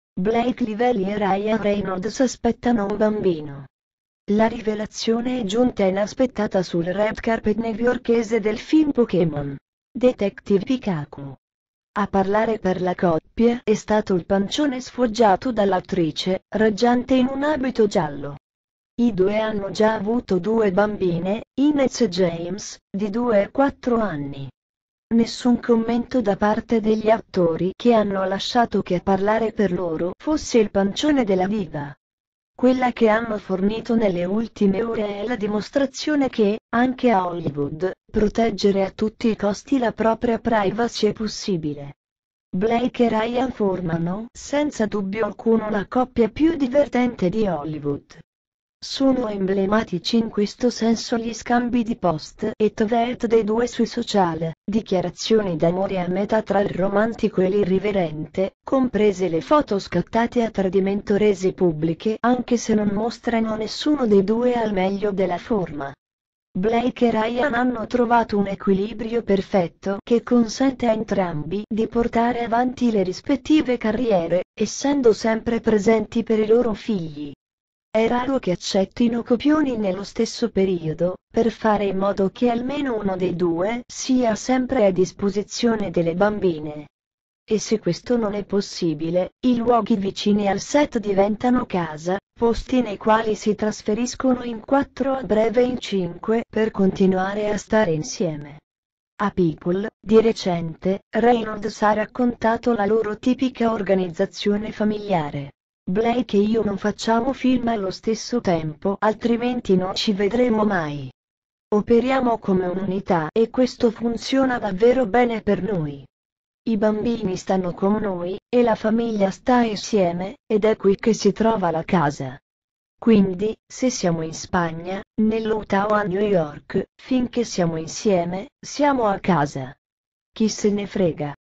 Blake Lively è incinta, aspetta il terzo figlio di Ryan Reynolds
Blake Lively e il marito Ryan Reynolds aspettano un altro bambino. La rivelazione è giunta inaspettata sul red carpet della première di Pokémon: Detective Pikachu, nel cui cast compare l’attore. Per la coppia si tratta del terzo figlio, dopo le nascite delle piccole Inez e James, di 2 e 4 anni.